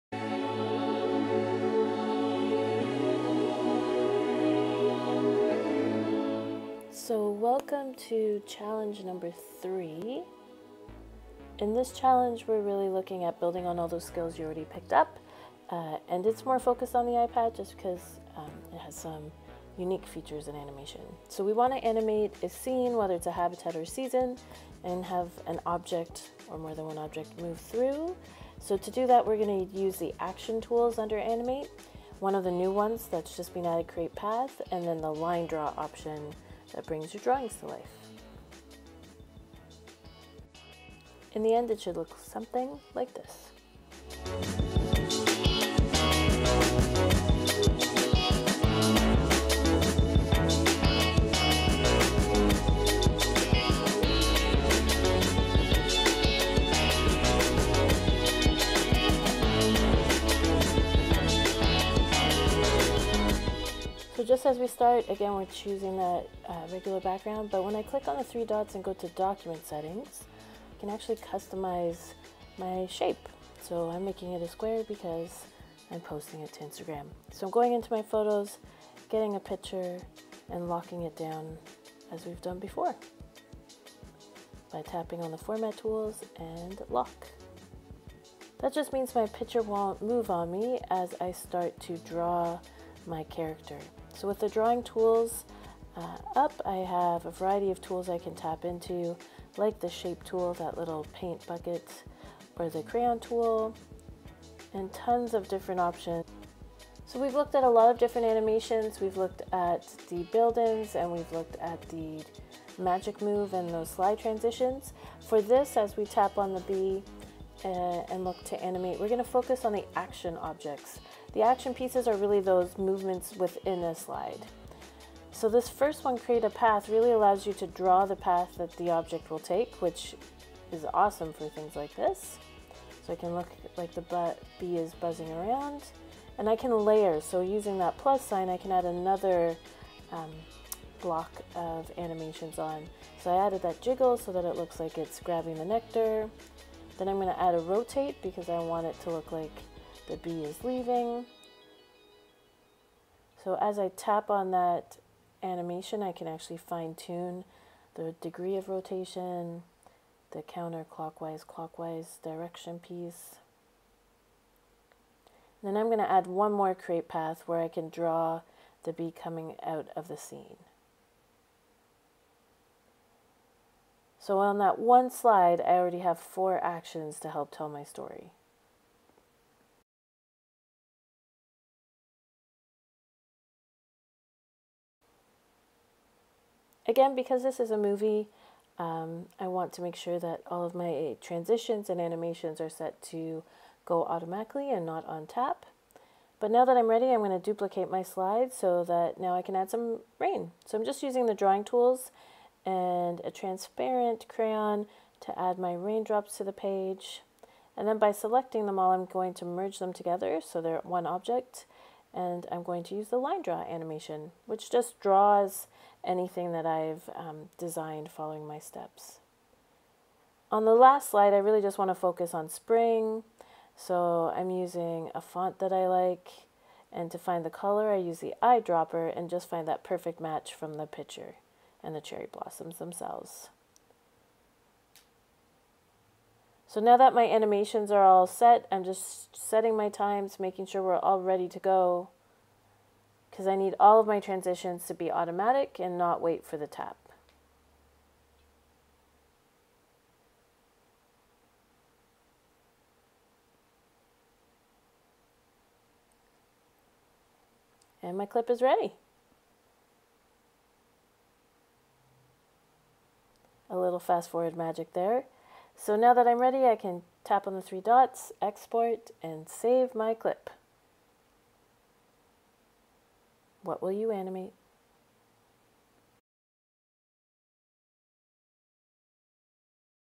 Animating with Keynote 101 - Challenge 3
Exploring how actions can enhance our Keynote animaitons.